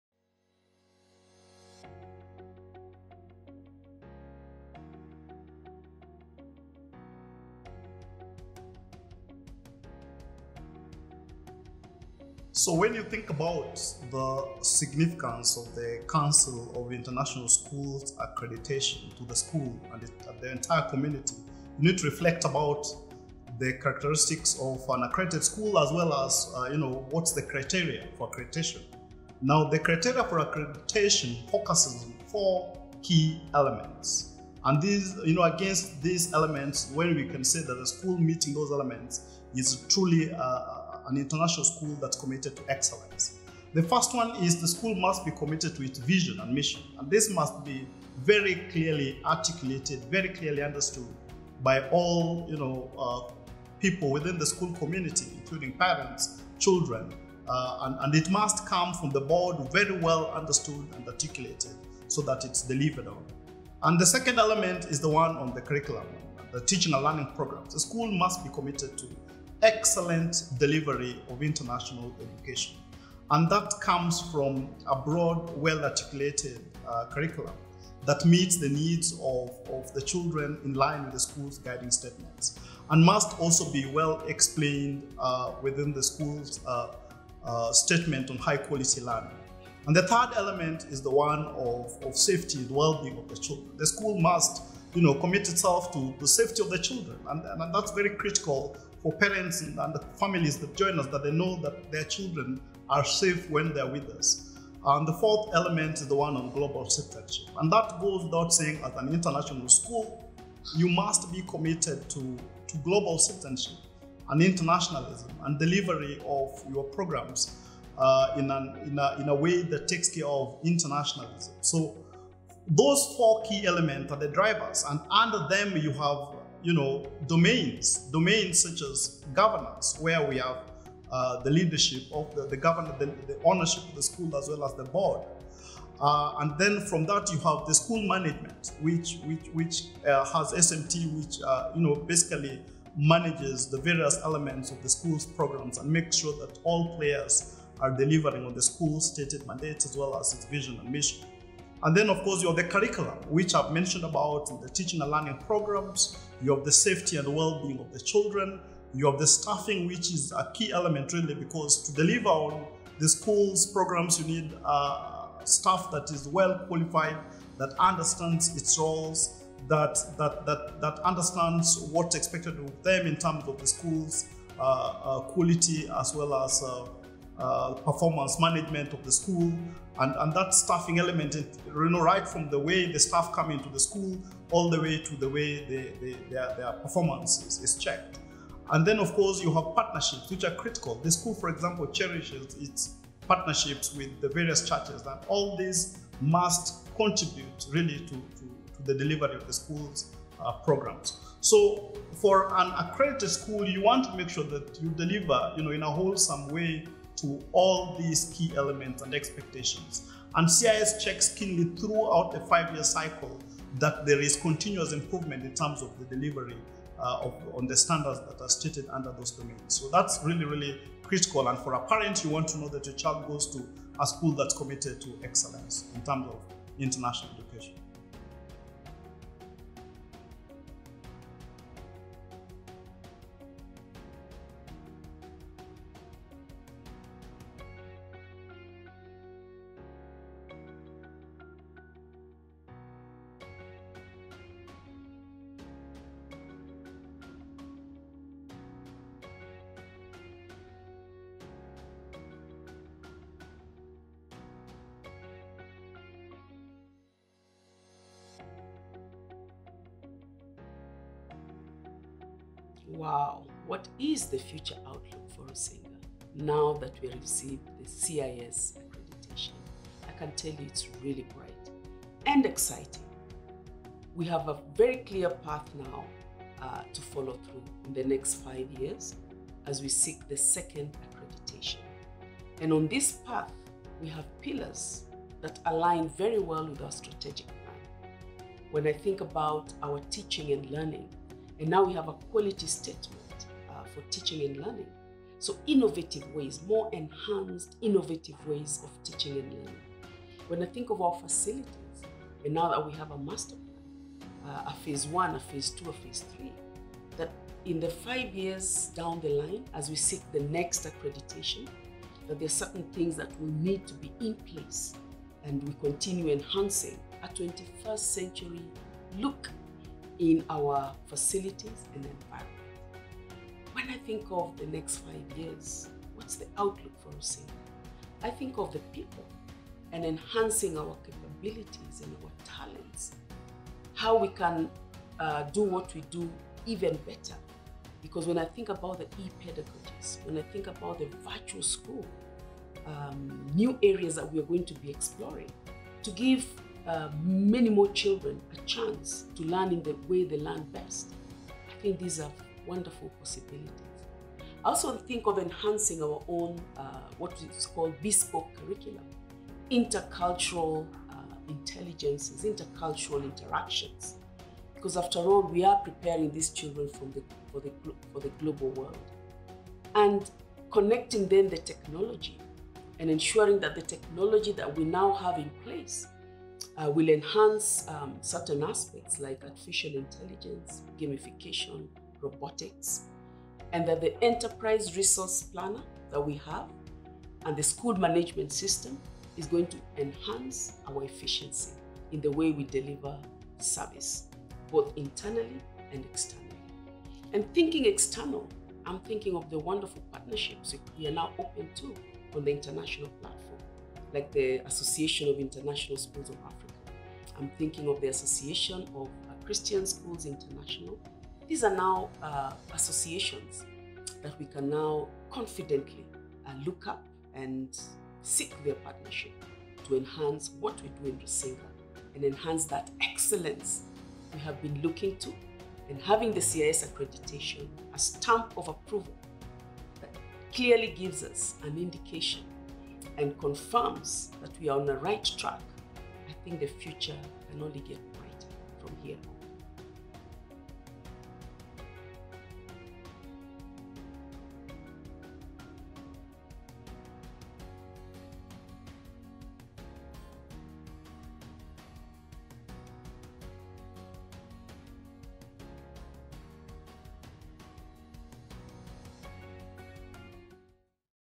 We Are Now A CIS Accredited School
Today, we have lots of good news to share. Drum rolls please... Rusinga Schools has achieved The Council of International Accreditation (CIS) Accreditation Award and we would like to share this exciting news with you. CIS, is a mark of recognition held exclusively by schools around the world that exemplify high quality standards in international education. What that means is our School and all that we do is internationally recognized! And if you watch and listen keenly to our headteacher Mrs. Echessa Kariuki and Mr. Juma Nyamori, Prep School Dean of Academic Programmes & CIS Accreditation Co-ordinator, you'll get a broader perspective of what this looks like for us, they explain it so well. Now this, is lots of good news under one significant accreditation. And since you all know we love to sing and dance, we welcome you to join us. Celebrate with us and show off your dance moves at this opportune time. A little jig to mark this great accomplishment will do! Celebrate with us! Rusingaschools CISaccreditation.